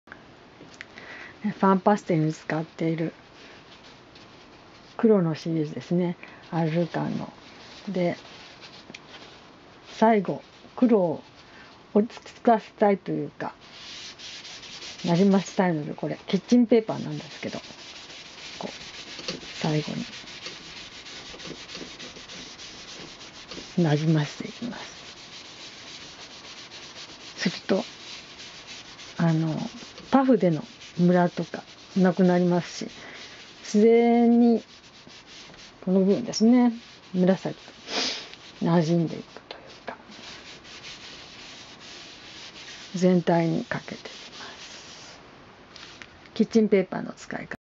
パンパステルのアルルカンの絵 黒色技法 Picture black technique of harlequin of PANPASTEL
パンパステルの黒色の可能性をアルルカンの絵の技法に見つけて最後の仕上げです。これはあくまでもモモ流です。参考の絵は、フクロウとアルルカンです。

A black possibility of PANPASTEL is found to the technique of harlequin's picture and the last finish. This is momoMsato style to the last. The picture of the reference is an owl and a harlequin.